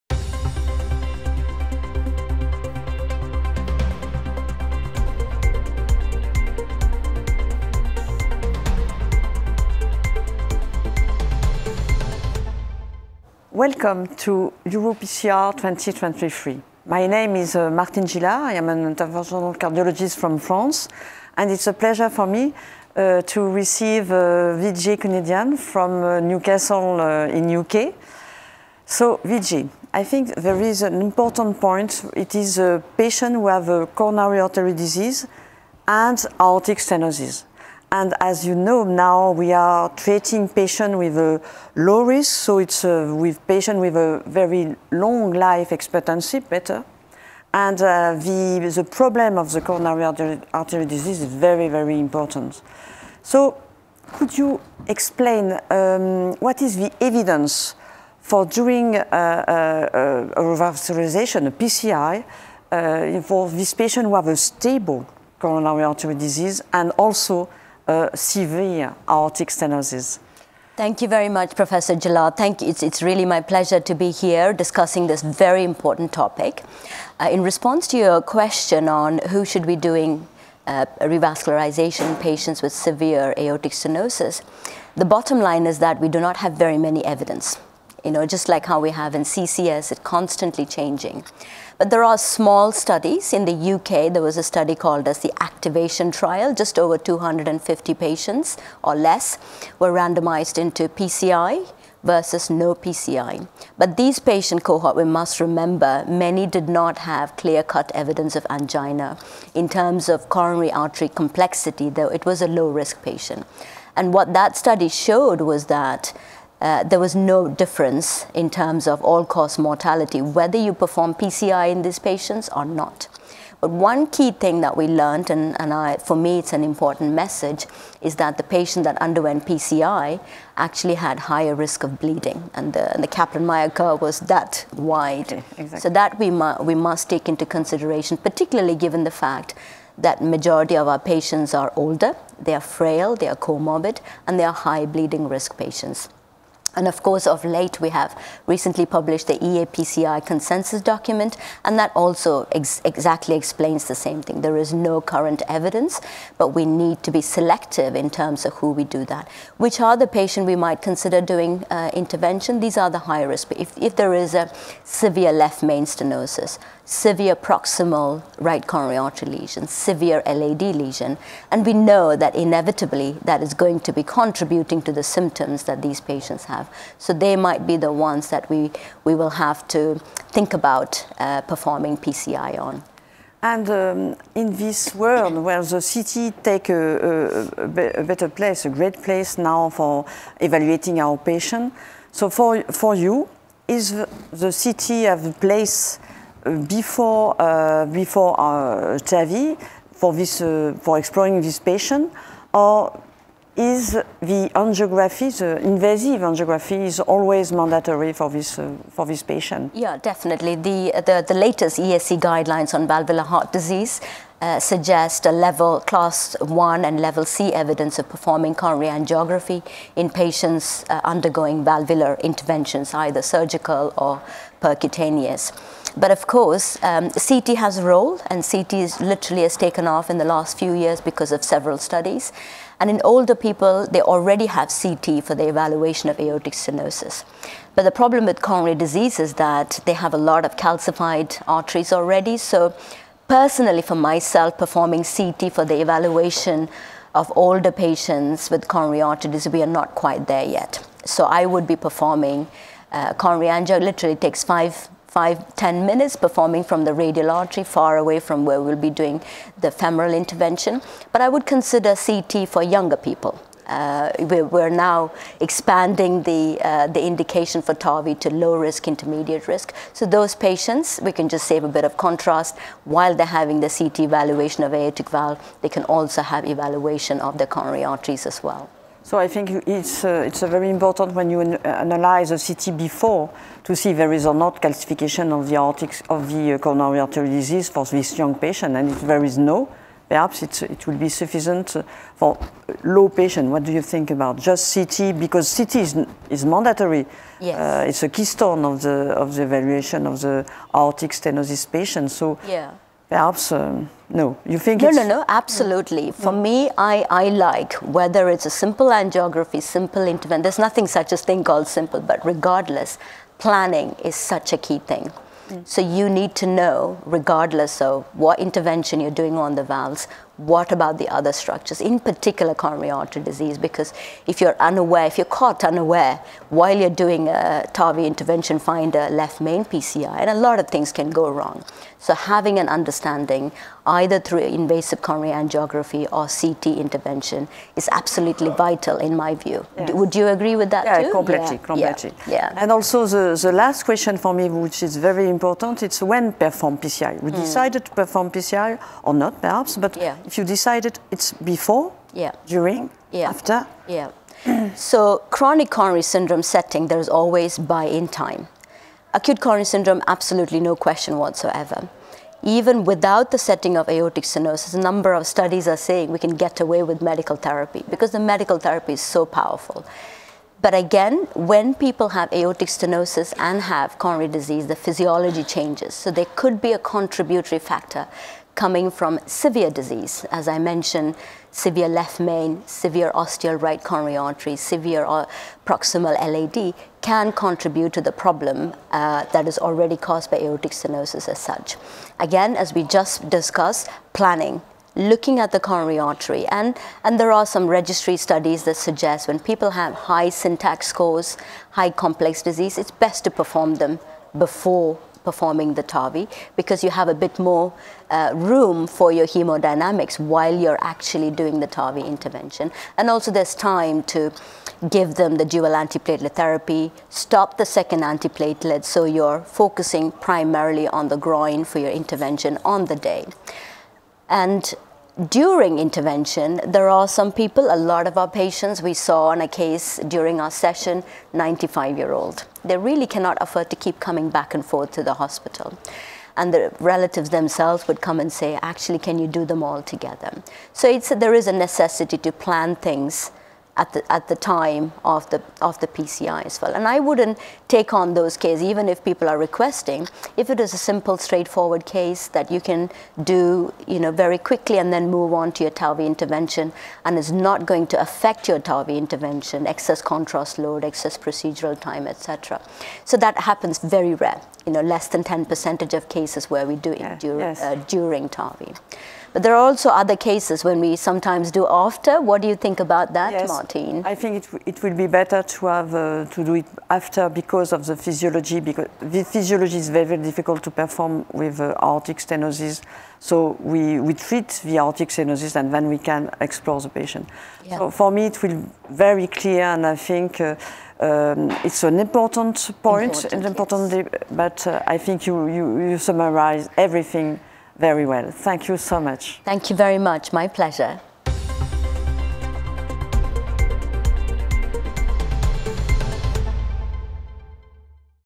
When should we perform PCI for stable CAD in aortic stenosis: pre, post, never? - EuroPCR 2023
What is the evidence for the need for PCI in patients with stable coronary artery disease and severe aortic stenosis? Is invasive angiography always mandatory for these patients? What about CT scans? When exactly should PCI be performed?

These are the questions Vijay Kunadian and Martine Gilard answer in this discussion about patients with stable CAD and severe AS.

The content of this video was filmed at EuroPCR 2023.